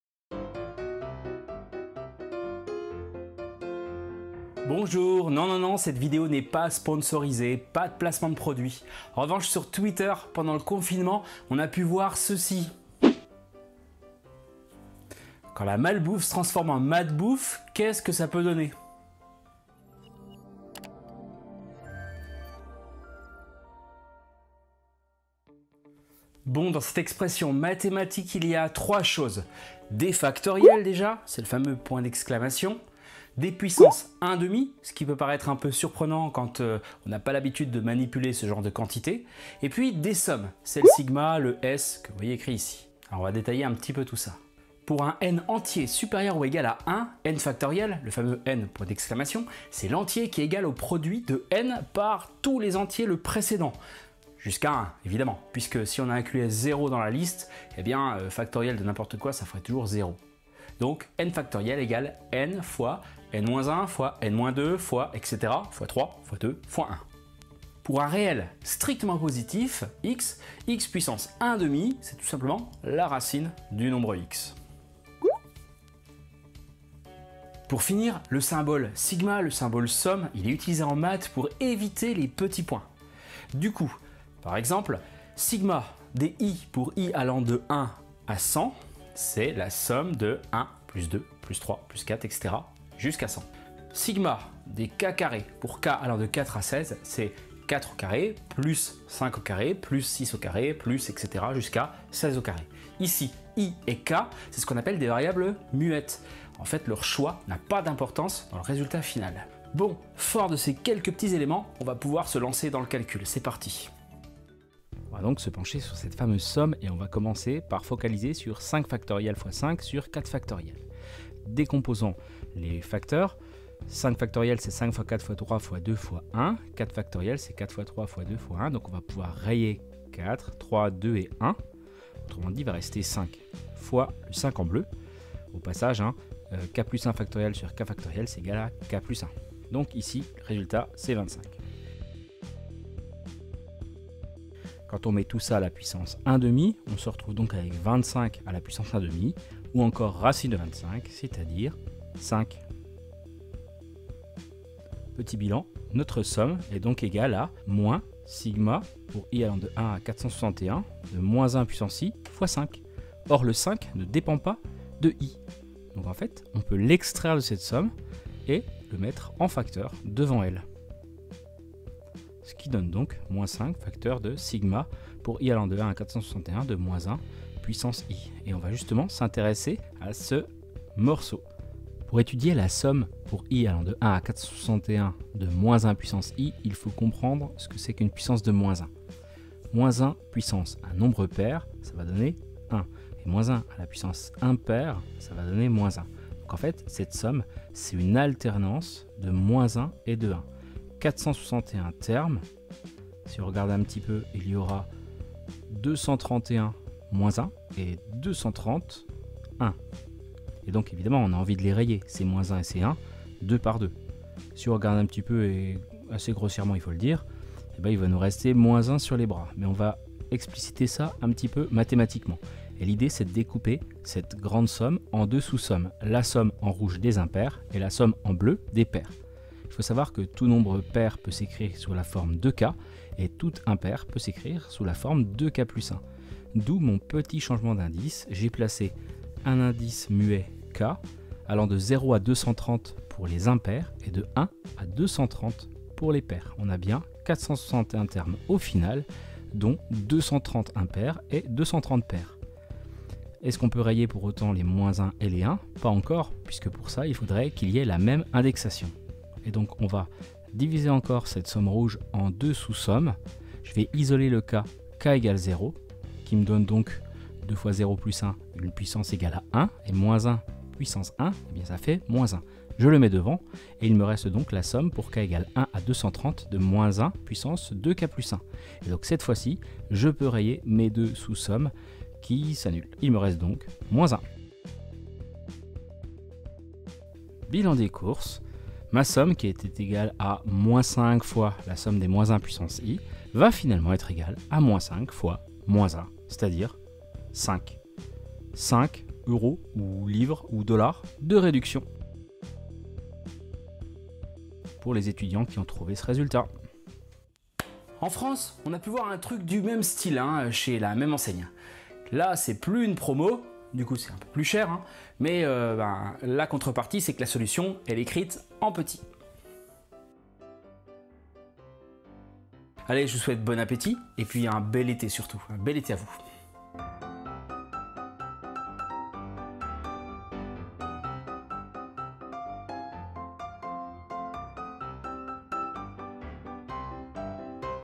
Les bienfaits de la Maths Bouffe - Vlog#19
Pendant le confinement, Burger King a proposé un petit challenge mathématique pour gagner une réduction... mais de combien, cette réduction ?